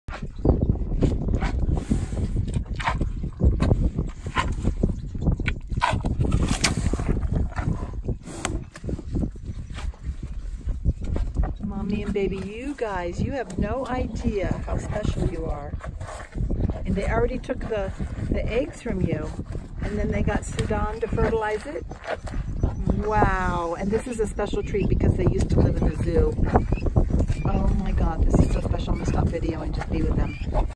Last 2 Northern White Rhinos in World - Up Close and Personal
Sudan, the world's last male northern white rhino, died in 2018 at age 45 causing researchers to focus on in vitro fertilization techniques using semen they had stored from male rhinos and eggs extracted from the two surviving females, Najin and Fatu. These 2 females live like wild rhinos most of the day but come to get carrots from tourists and donors like me for an hour a day at the Ol Pejeta Rhino Rehab Center. My response being this close to this semi-wild rhino when she looked me straight in the eye? Tears of gratitude.